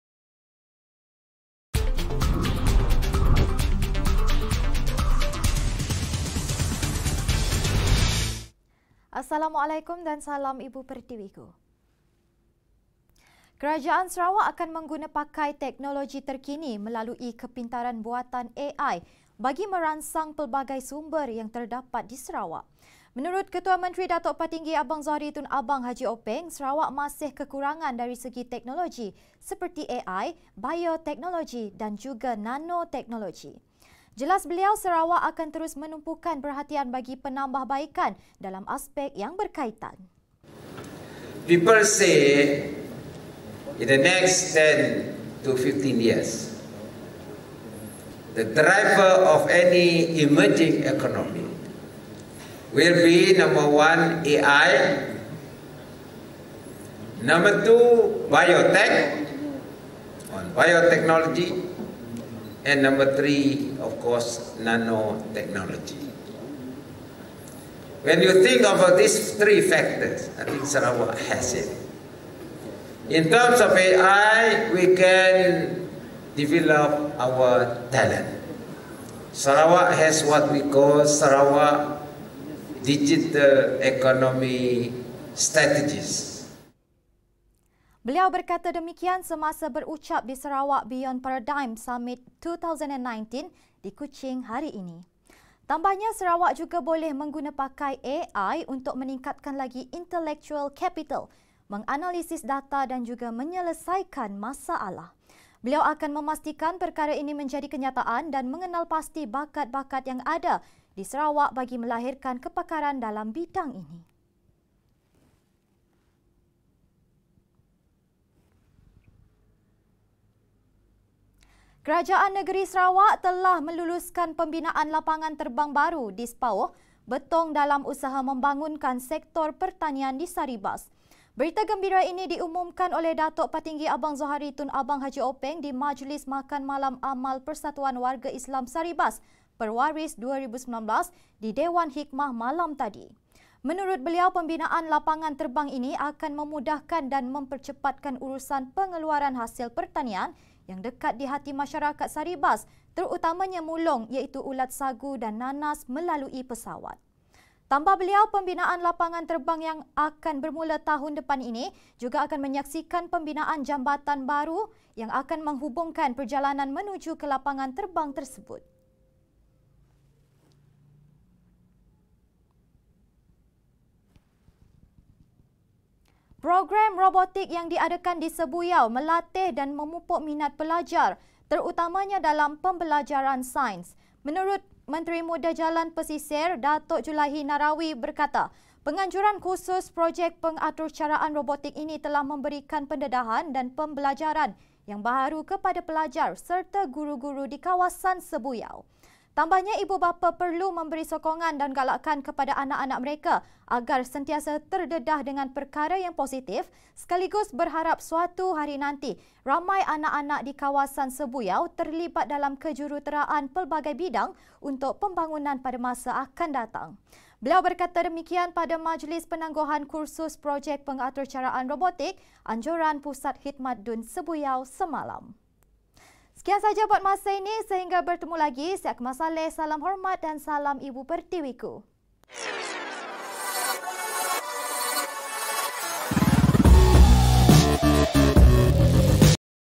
Sarawak Akan Mengguna Pakai Teknologi Terkini Melalu AI
1. Sarawak Akan Mengguna Pakai Teknologi Terkini Melalu AI
2. Lapangan Terbang Baharu Di Spaoh Rancakkan Sektor Pertanian Di Saribas
3. Program Robotik Pupuk Semangat Pelajar Dalam Pembelajaran Sains